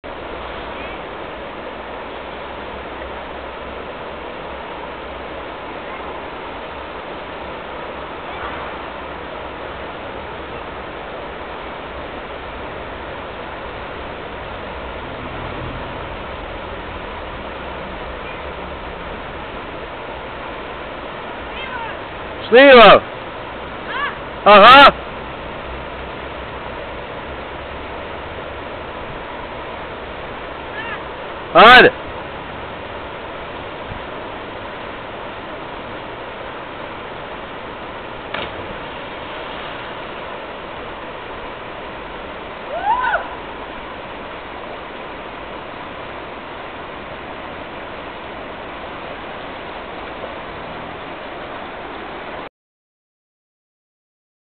Dzeno hoppar !!!
Jag hoppar första gången från en bro som är typ 10m hög i Bosnien/Banja Luka ''Viseci most u seheru'' (Det är den bron)